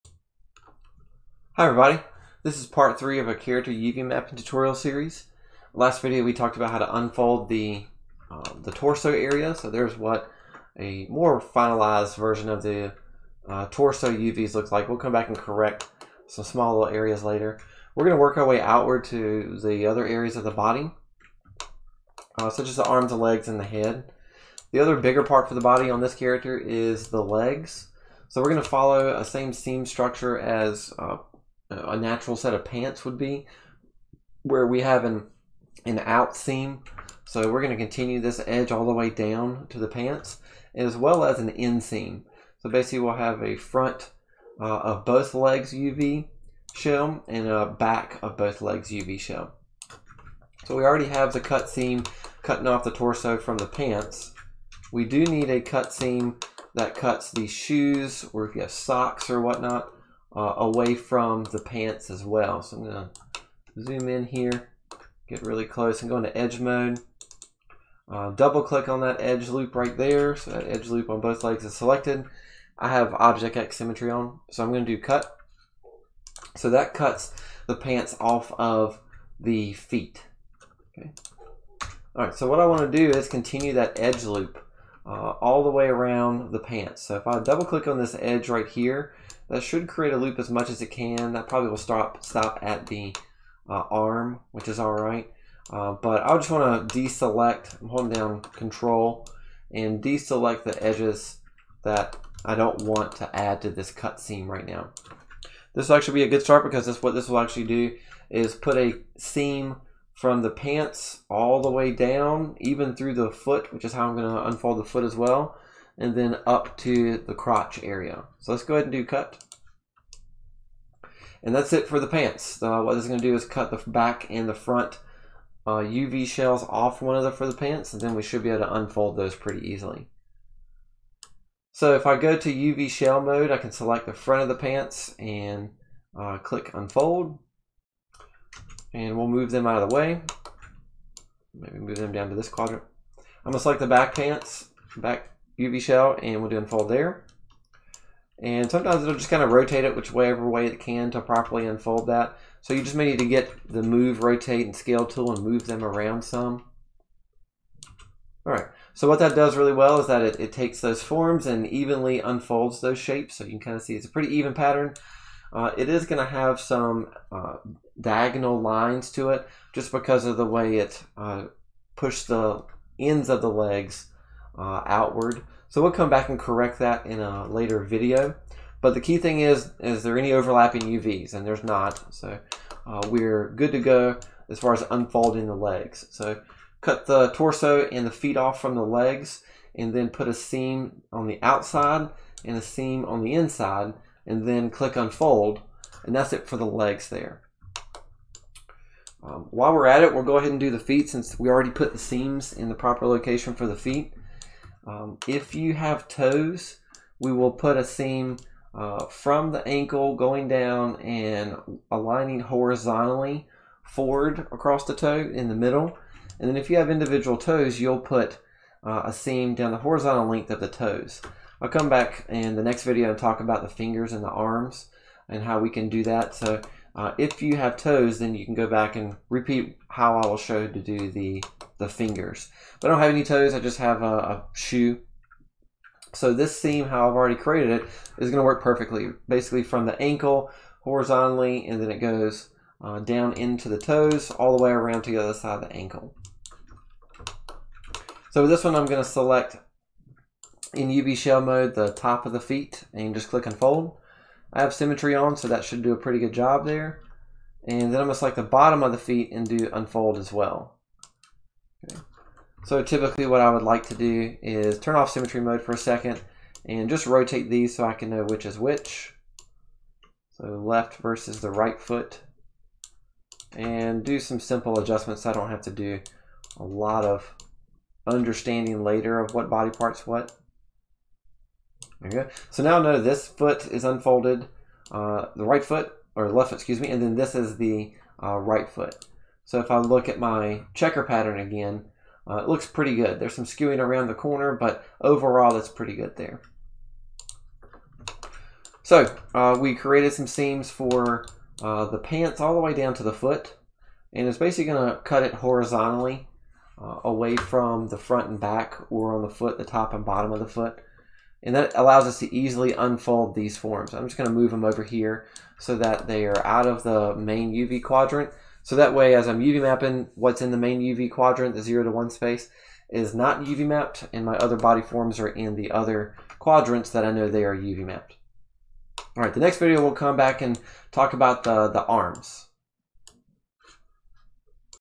Character UV Mapping Part 03 Legs And Feet UVs
This is part 3 in the character UV mapping tutorial series. In part 3, I demonstrate how to cut the UV edges for the legs and feet and unfold these forms properly.